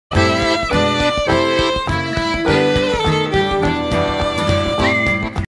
4/4 Delicious Flammekueche 'Flamm' straight out of oven ! - Night market in Montendre
Restaurant Cave La Quincaillerie @ marchés nocturnes de Montendre